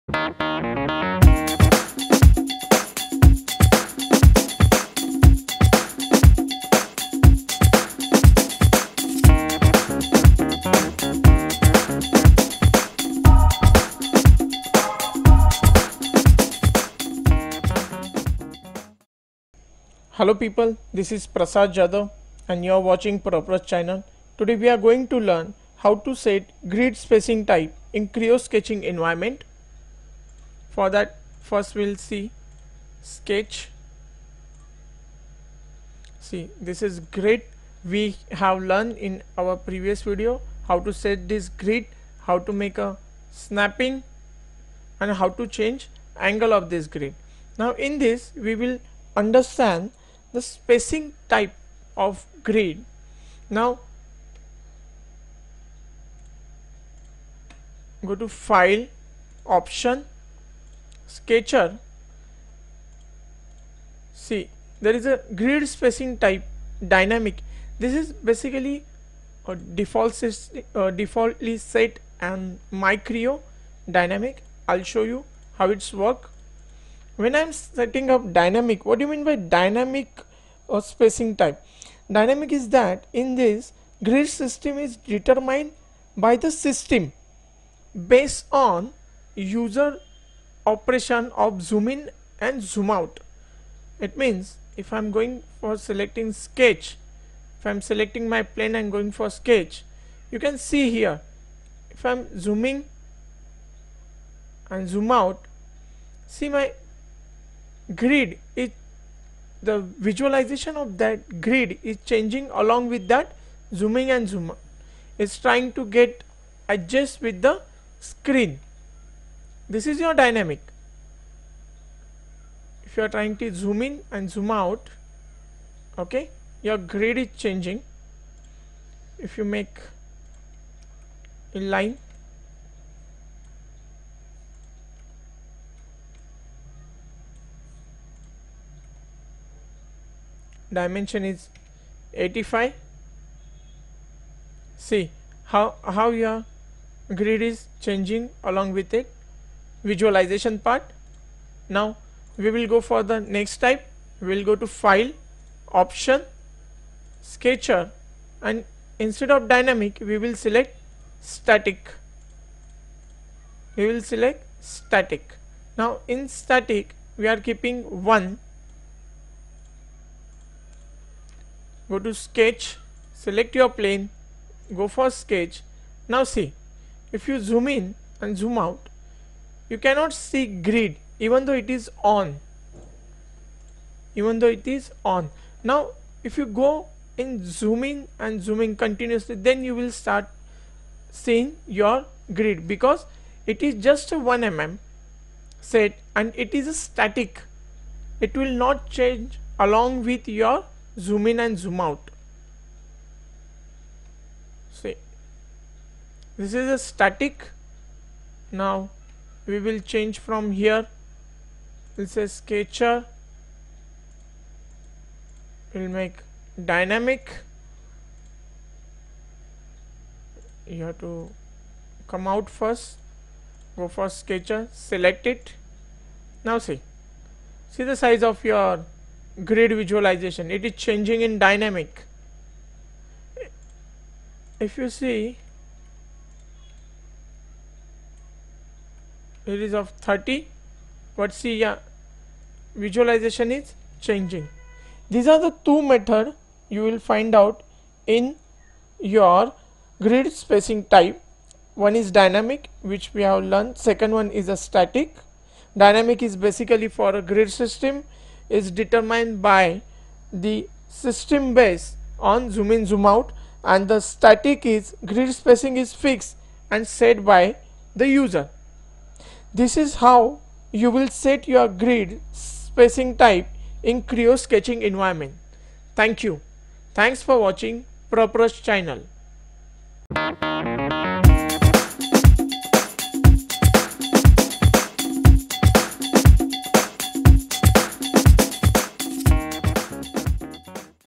Creo - Set Grid Spacing Type in Sketching Environment
In this session, we will learn how to set grid spacing in Creo-parametric sketching environment